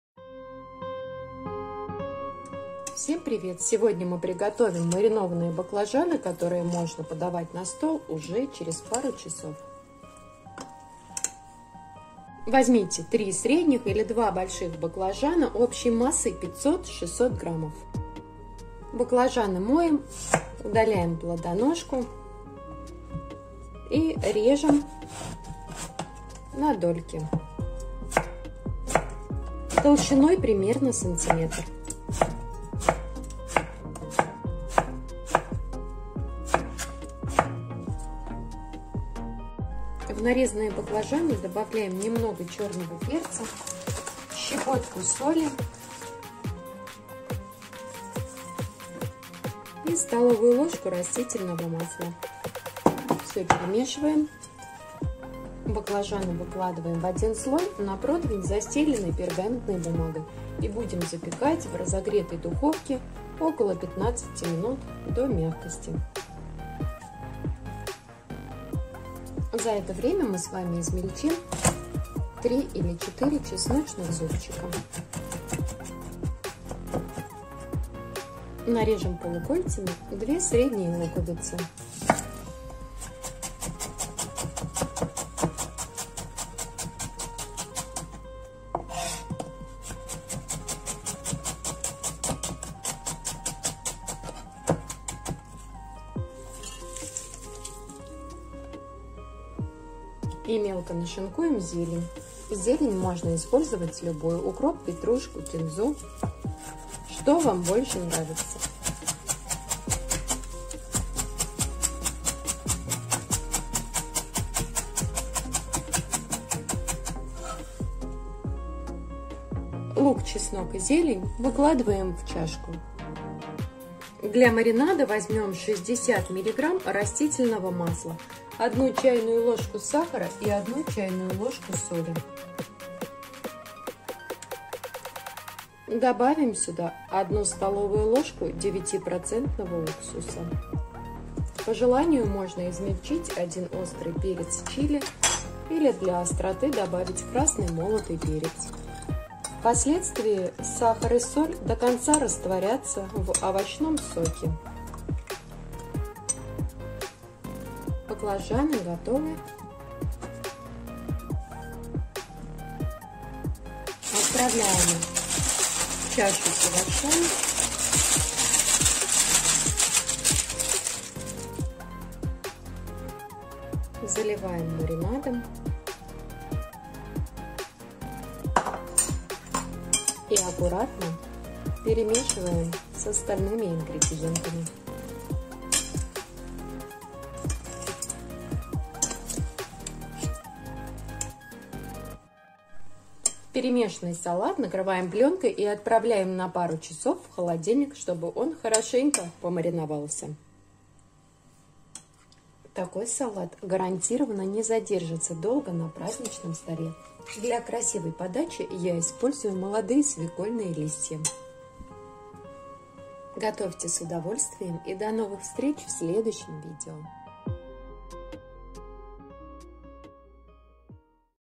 Маринованные баклажаны с луком и чесноком. Простой рецепт за 20 минут!
Маринованные баклажаны с луком и чесноком – простая, популярная и востребованная заготовка из серии рецептов «На скорую руку» Вкус идеальный - ни чего лишнего !

Маринованные баклажаны с луком и чесноком – простая, популярная и востребованная заготовка из серии рецептов «На скорую руку»

Нам понадобится:
Баклажаны - 3 шт.( 600г),
Лук репчатый-1-2 шт.,
Чеснок  - 3-4 зуб.,
Петрушка -20г ,
Масло подсолнечное (без запаха)-60 мл.,
Уксус 9% - 25 мл. (1 ст.л.),
Сахар - 1 ч.л.,
Соль  - ч.л.

Баклажаны запекать при температуре 220градусов  15 минут

Смотрите мой канал
в ДЗЕН :